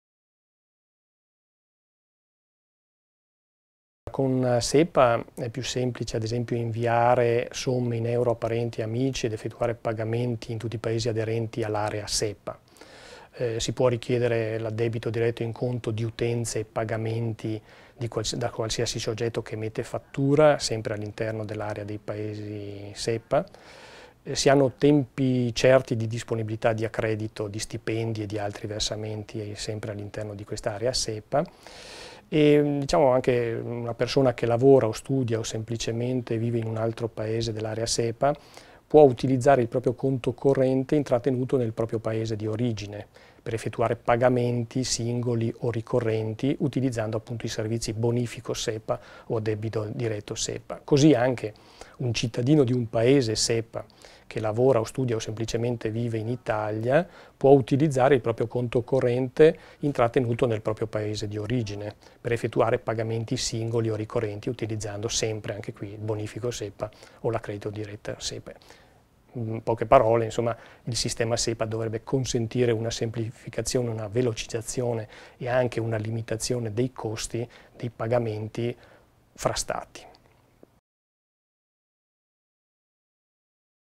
Quali sono i vantaggi di SEPA?
La SEPA (Single Euro Payments Area) è l’area unica in cui i cittadini, le imprese e gli enti, possono eseguire e ricevere pagamenti in Euro, all’interno dei confini nazionali e tra i paesi diversi che compongono l’area SEPA con condizioni di base, diritti ed obblighi uniformi tra i paesi stessi. Paolo Guerriero del Centro Tutela Consumatori di Bolzano spiega al team dell’Euregio i dettagli.